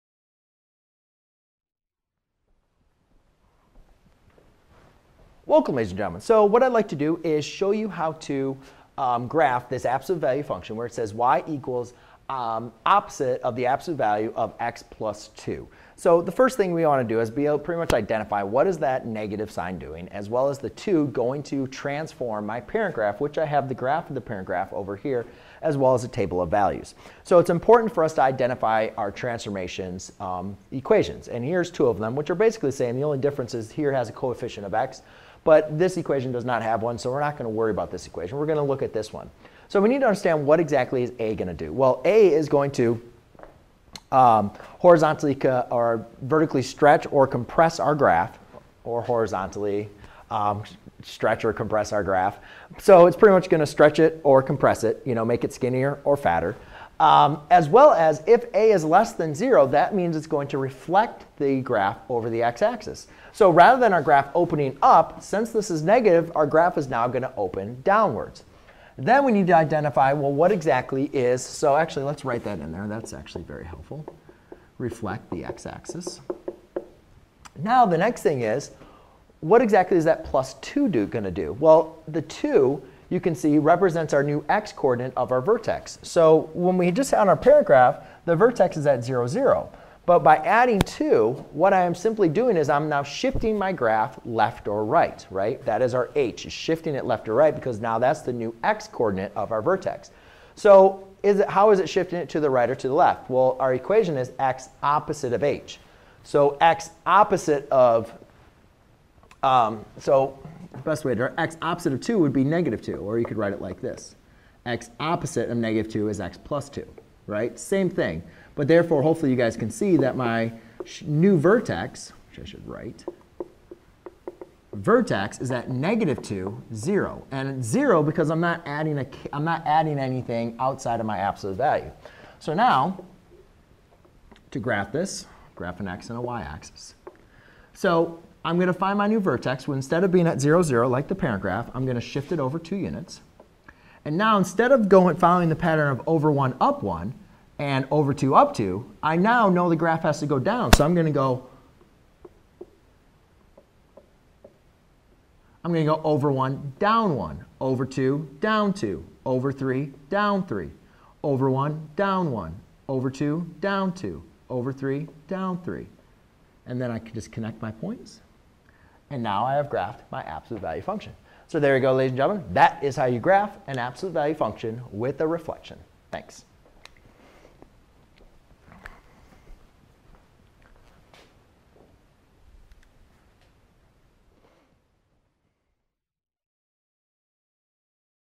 Graph a absolute value equation with a reflection and horizontal translation
👉 Learn about graphing absolute value equations. An absolute value equation is an equation having the absolute value sign and the value of the equation is always positive. The graph of the parent function of an absolute value equation is a v-shaped graph starting from the origin above the x-axis and rising both sides of the y-axis and is symmetrical to the y-axis.

To graph an absolute value equation, we first graph the parent function of the absolute value equation and we then apply the necessary transformation(s) to the graph of the parent function to obtain the required graph of the absolute value equation.